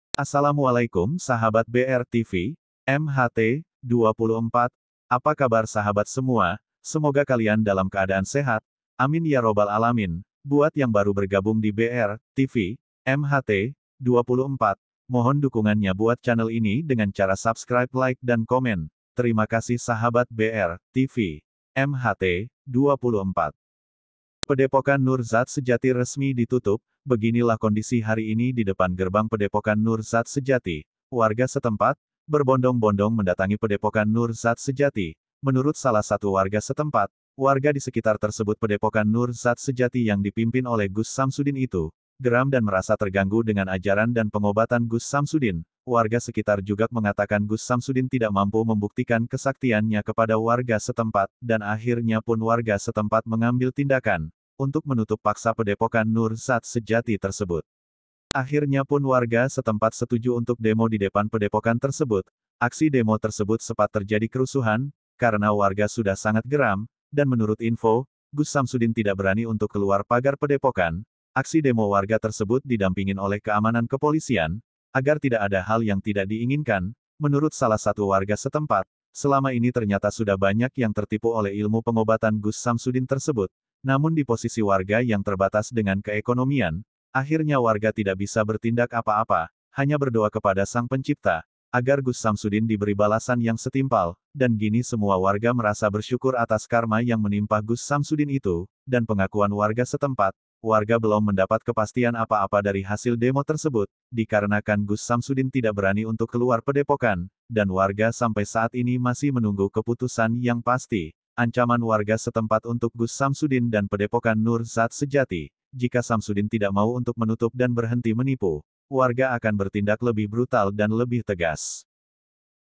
geger !! demo besar besaran di pedepokan nur dzat sejati | warga paksa tutup pedepokan gus samsudin
Berita terbaru hari ini, pedepokan nur dzat sejati pimpinan gus Samsudin di grebek masa, di demo masa atau warga setempat.